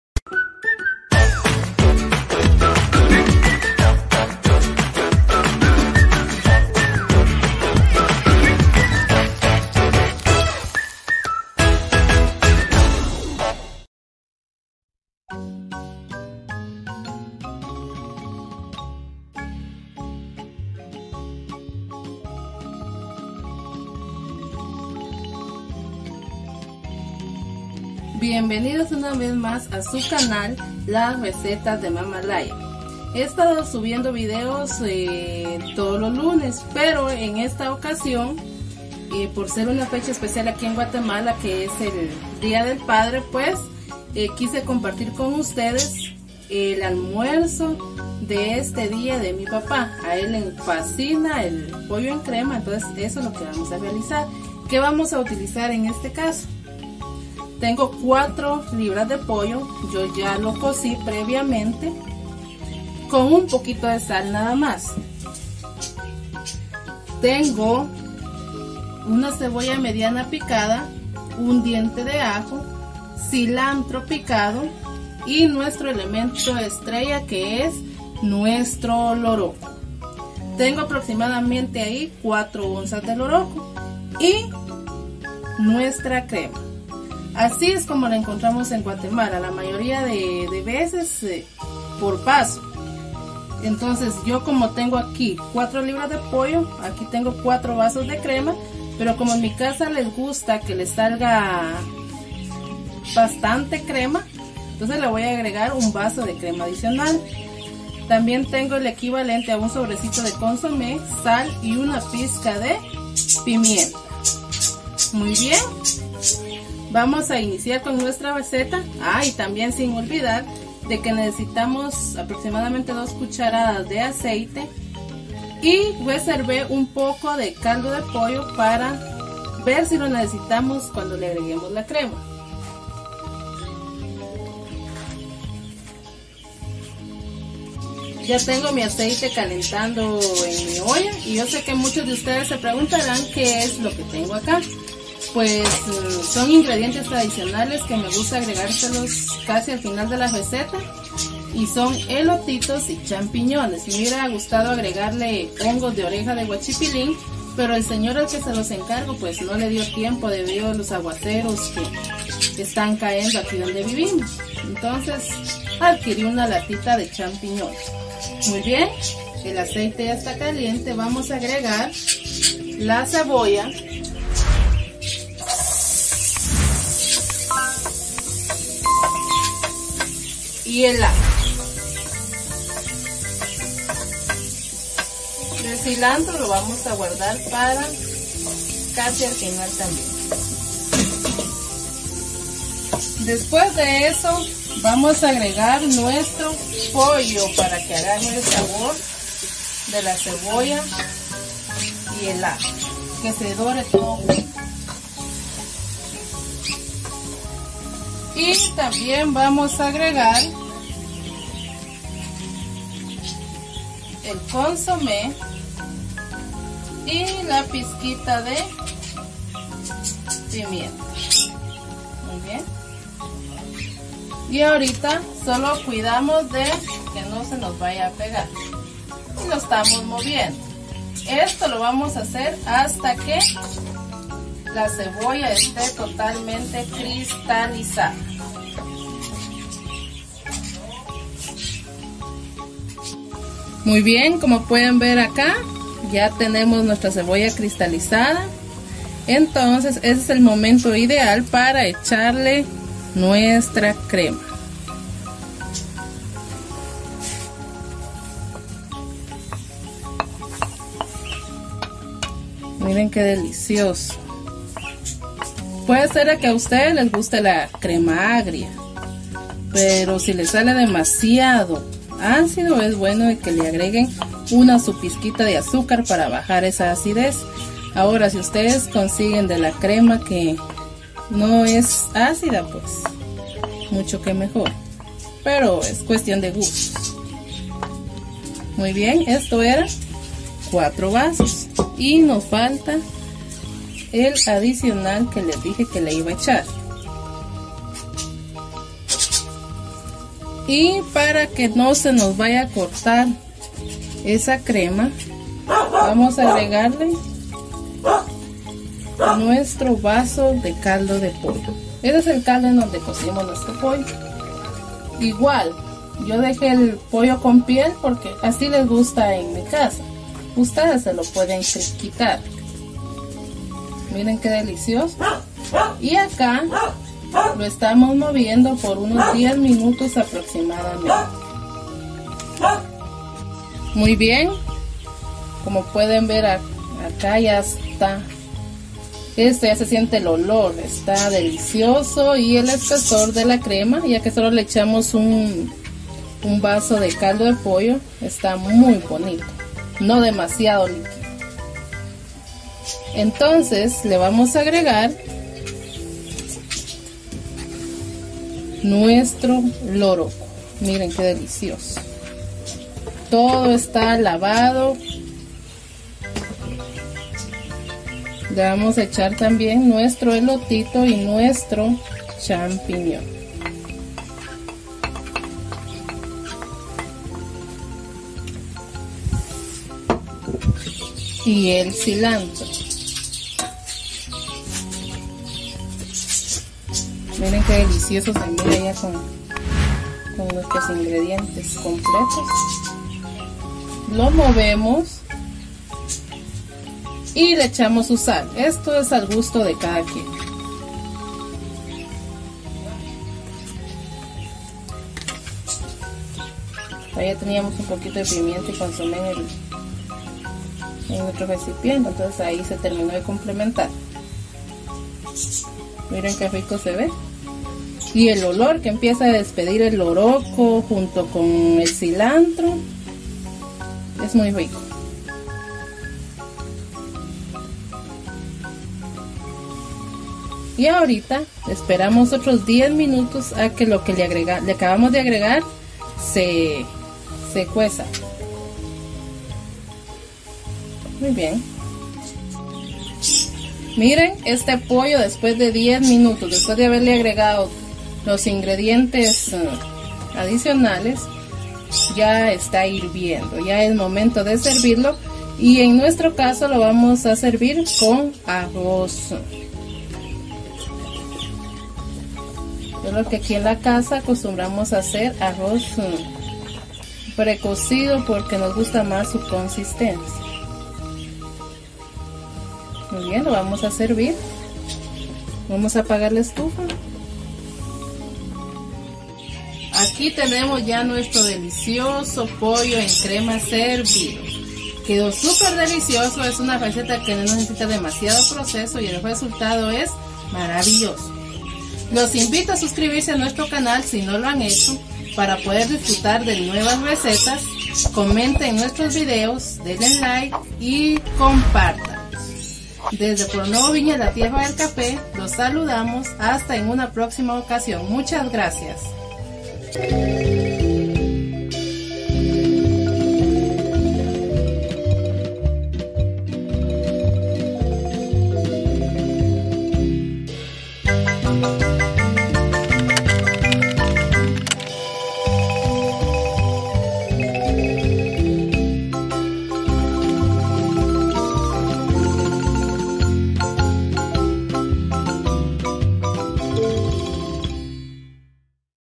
POLLO EN CREMA CON LOROCO EN EL DÍA DEL PADRE | Las Recetas de Mama Laya
Aquí tienes una Receta Facil de POLLO EN CREMA CON LOROCO que realicé para mi papá en su día.  Es un delicioso platillo hecho a base de caldo de pollo, crema y loroco; el loroco le da un toque muy especial. Además de ser una receta chapina facil de hacer, espero lo disfruten.